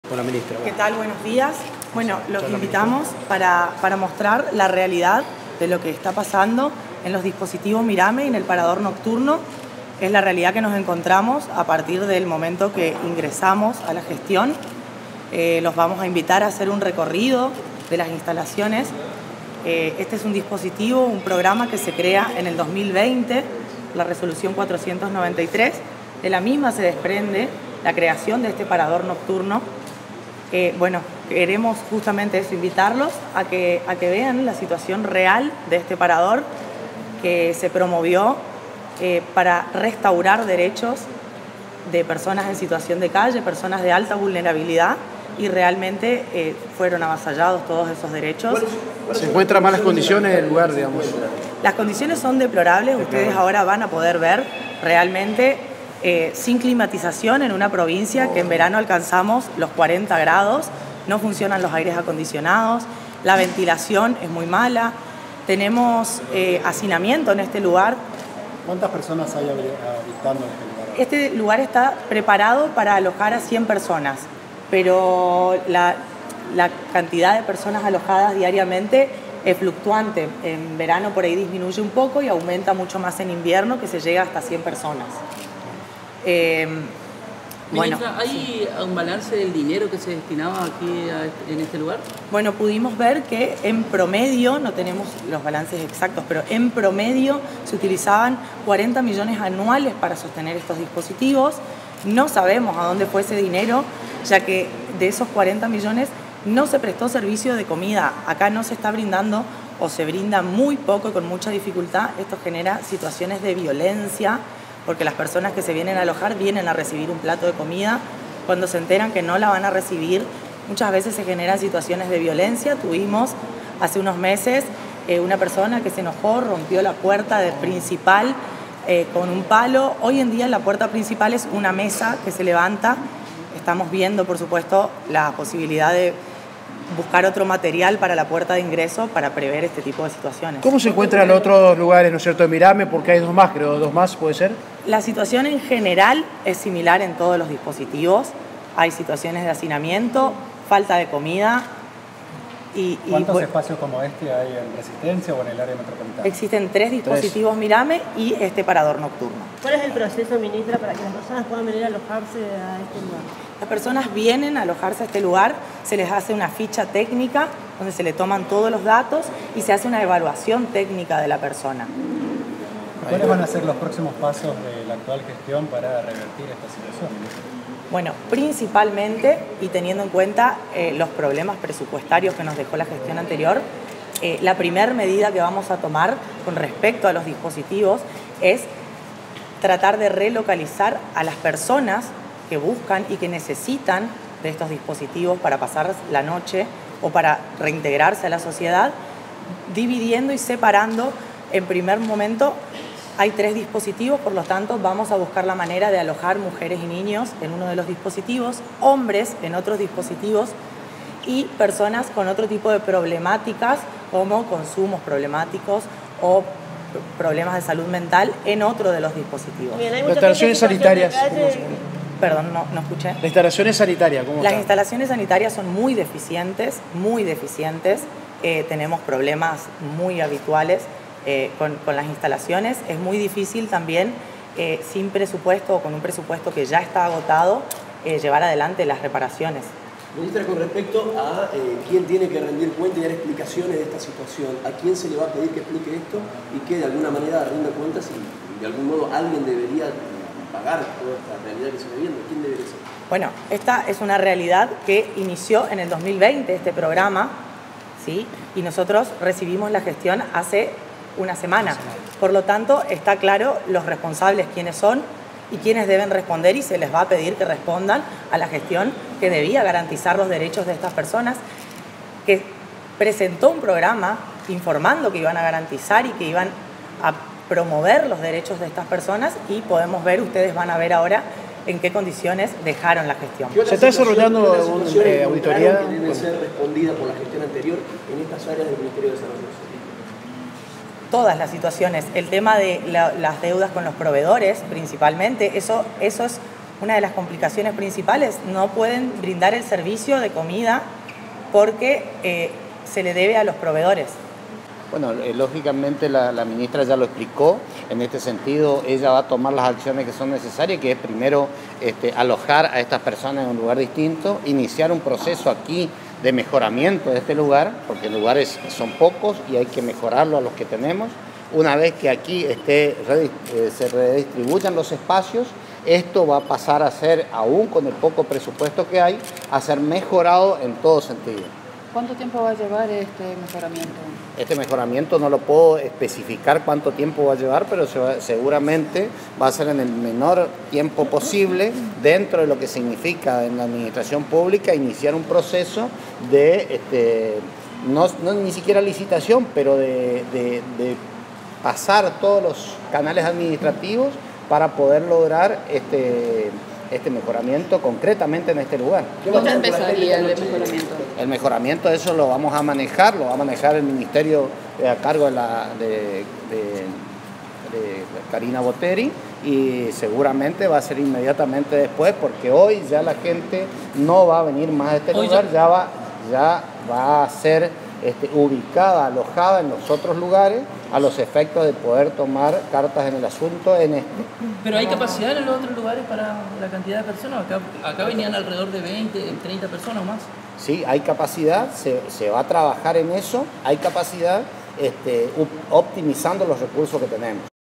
El programa “Mírame” de contención social funcionaba en un estado deplorable
La ministra de Desarrollo Humano Carina Botteri Disoff constató este martes, junto al ministro de Gobierno, Justicia, Trabajo y Derechos Humanos Jorge Gómez, el estado del edificio del dispositivo Mírame, ubicado en avenida 25 de Mayo 855, de Resistencia.* “El estado es deplorable, fueron creados para restaurar derechos de las personas en situación de calle y *lo único que hicieron fue avasallarlos”,* aseguró Botteri Disoff. Los funcionarios detectaron falta de mantenimiento, de climatización y hacinamiento.

Ante ello, adelantó que la primera medida para atender a las personas que lo necesitan es relocalizarlas: mujeres y niños irán a un lugar, hombres a otro y las personas con problemas de adicciones a otros.

“Este programa inició en 2020 y nosotros recibimos la gestión hace una semana por lo tanto está claro quienes deben responder y a quien le vamos a pedir explicaciones”, expresó Botteri Disoff.

“No tienen climatización, ya no funcionan los aires, la ventilación es mala y las personas están en condiciones de hacinamiento”, detalló la ministra. Además, indicó que el servicio de comida es deficiente, por lo que genera situaciones conflictivas con quienes acuden para acceder a un alimento. “Uno de los mayores problemas es que hay una gran deuda con los proveedores de comida, por lo cual no se brinda la atención necesaria”, resaltó.

Actualmente hay tres dispositivos Mirame. “La situación es general en todos y será muy difícil sin presupuesto o con un presupuesto ya agotado empezar la reparación”, afirmó la ministra.

Optimizar los recursos para garantizar derechos

Gómez detalló que, si bien se comenzó el proceso de mejoramiento, no es posible especificar el tiempo que llevará ya que se debe cumplir los canales administrativos. “A partir de hoy martes, las personas ya no se alojarán en ese lugar, trataremos de optimizar los recursos para garantizar los derechos”, aseguró.

Este medio se sostiene gracias a la colaboración de nuestros lectores.